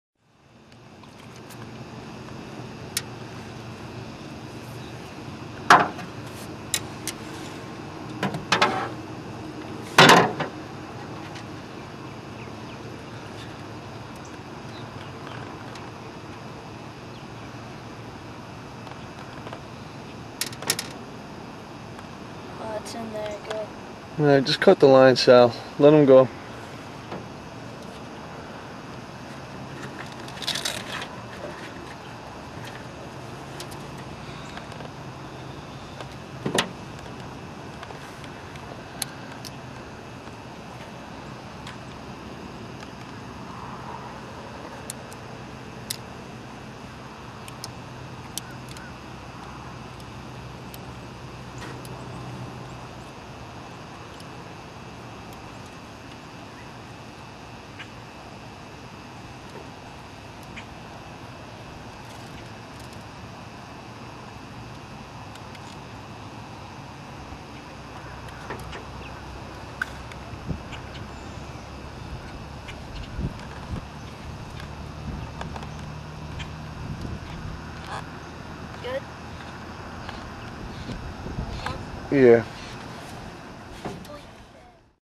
Largemouth Bass- Freshwater fish Long Island
This Largemouth Bass was caught out of Peconic Lake in Riverhead.